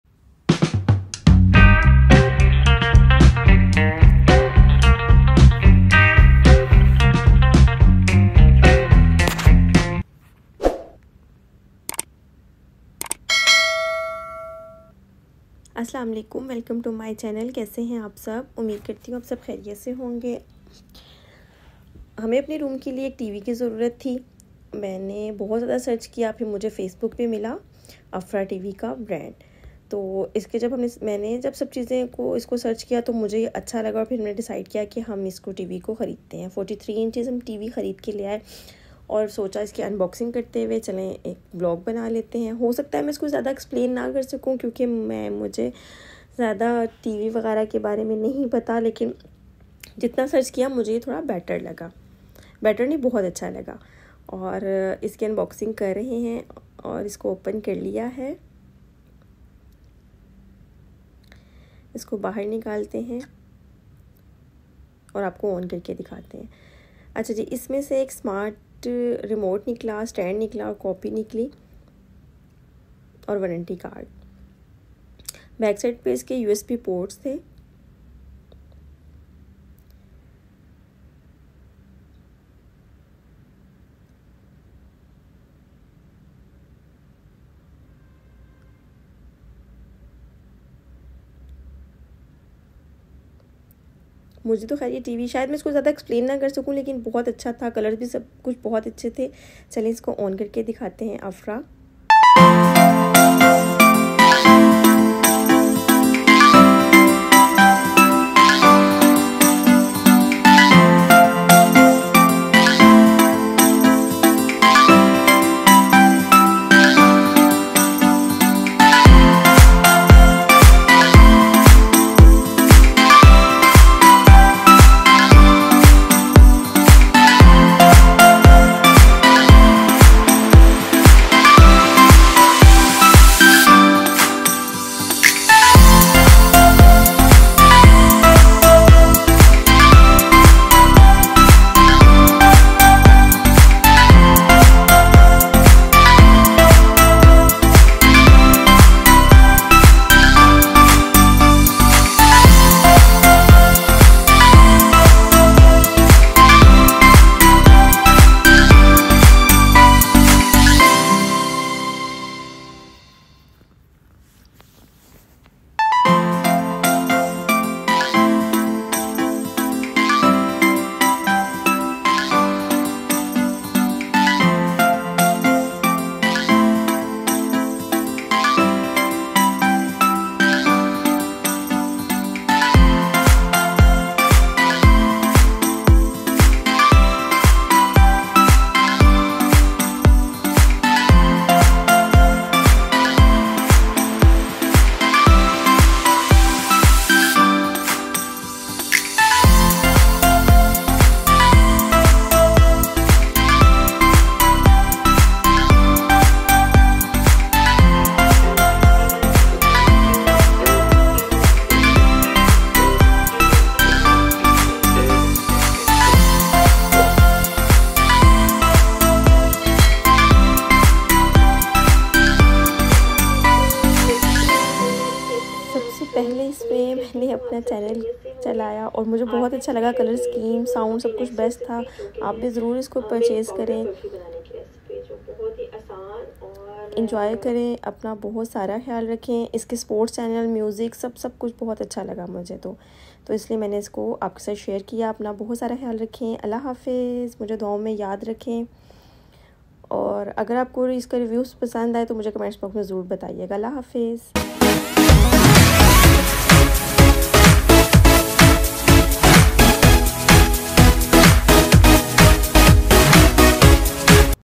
Unboxing AFRA LED TV 43 Inches | AFRA Review Best Smart Tv | Best Look Best Budget Smart Tv 👍
smarttv unboxing afrasmarttv besttv# best price good quality best sound nice colour best led latest lounch